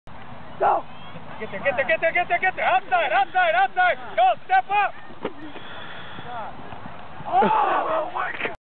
Youth Football Oni steps up in the pocket
Not bad for for an 11 yr old